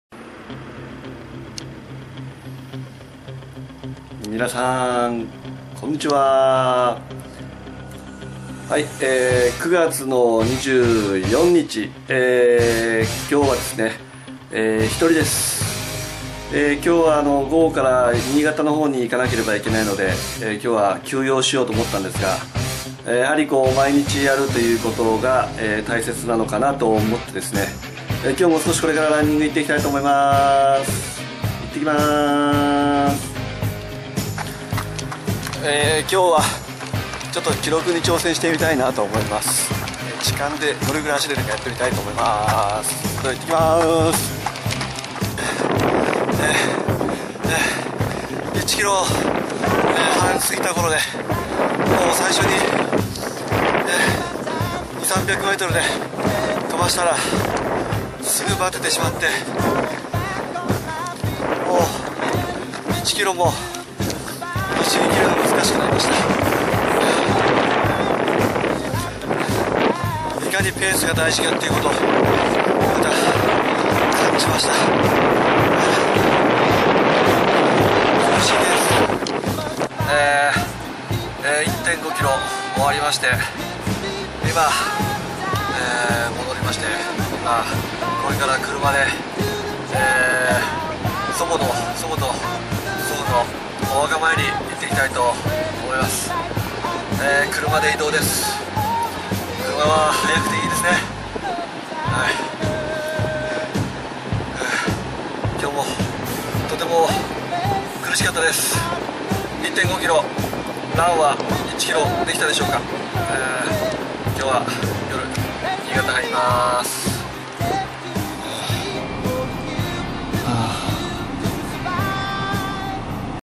【古市佳央】ホノルルマラソンへの道 メデューサが！ 2013年9月24日
マラソンが大嫌いな古市佳央がホノルルマラソンにチャレンジする姿を追いかけるシリーズ。

ホノルルマラソンへの道！６３４（ムサシ）プロジェクト始動！（目標６時間３４分完走）

2013年9月5日からスタートし、最初は１キロも苦しくて走れなかった所からのスタートが徐々に走れるようになっていく様を描いていきます。

今回は新潟に行く前の一人でのランニング１・５キロ！

胸元にはメデューサが！

６時間３４分で完走を目指す634（ムサシ）プロジェクト始動！